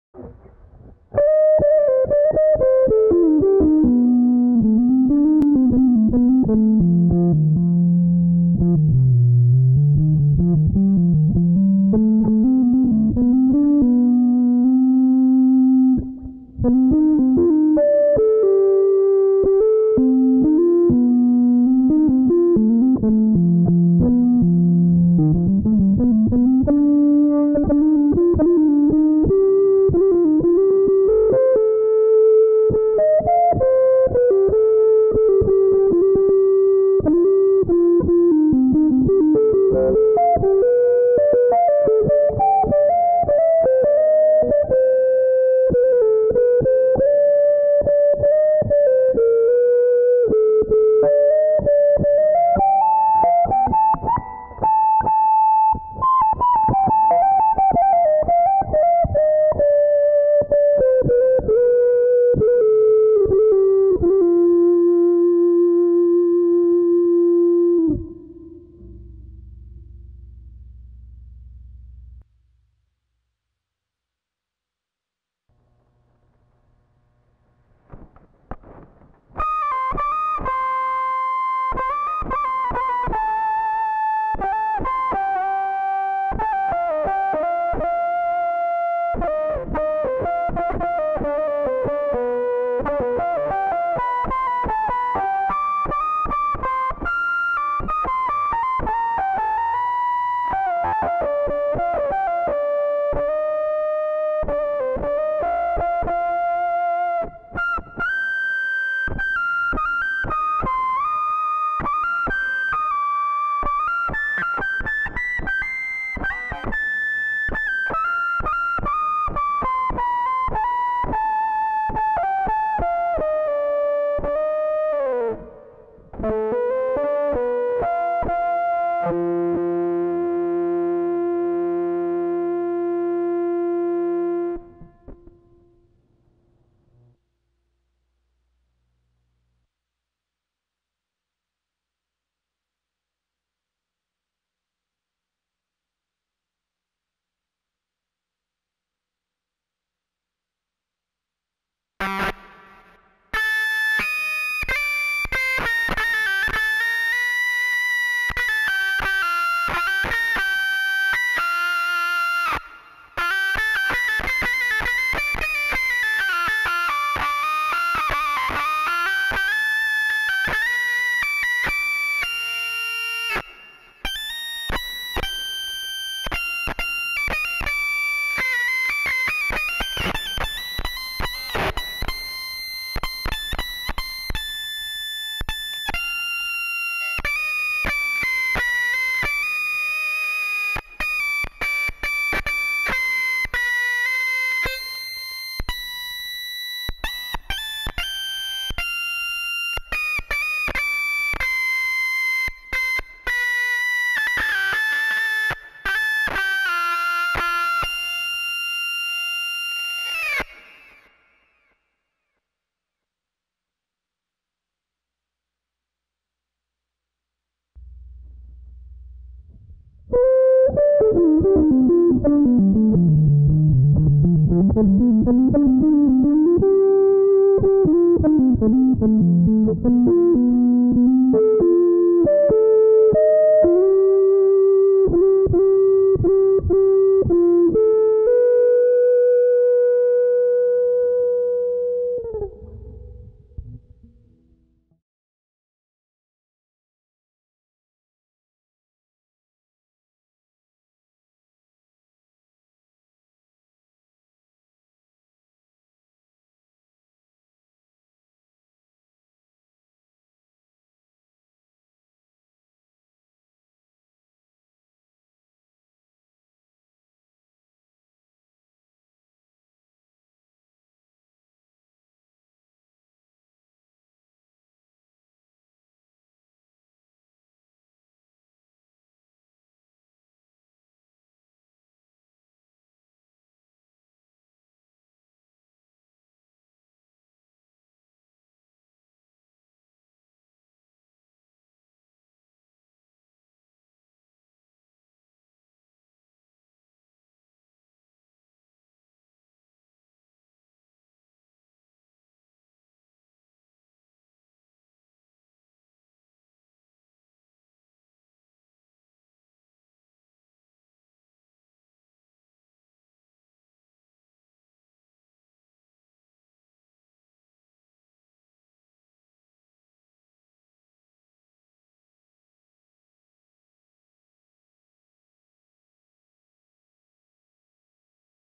Guitar Frequency 'Quadrupler' Circuit on Breadboard
In retrospect, I think Guitar Frequency Quadrupler is a misnomer, as the circuit can process any signal within the normal guitar range (82~1320Hz). The input signal is first amplified, compressed and filtered. The complicated part is leaving the fundamentals untouched and attenuating partials at least by 12dB, over the entire frequency range of interest, this requires a frequency tracking filter and a set of well tuned band reject filters, switched according the right frequencies at the right moment. The effort in developing and tuning the switched filters is recompensed when we hear a quasi sine wave at the filters output, maintaining a nice constant amplitude over the guitar range. The frequency multiplication circuits that follows requires this clean signal in order to work best. The multiplication is made in two distinct steps, first a precision full wave rectifier doubles the signal frequency, its output is then transformed into a square wave and sent to a second frequency doubler, this time made with CMOS logic. This final frequency quadrupled signal can then be used as a two octaves higher transposition effect, can be frequency divided in order to get a different transpositions: divide by three still provides an interval above the initial signal fundamental, divide by five, six or seven will be within the first octave bellow fundamental, and etc... Another use of the quadrupled frequency signal is to derive a frequency to voltage converter using higher cutoff filters, and hence getting faster frequency to voltage conversion with less ripple.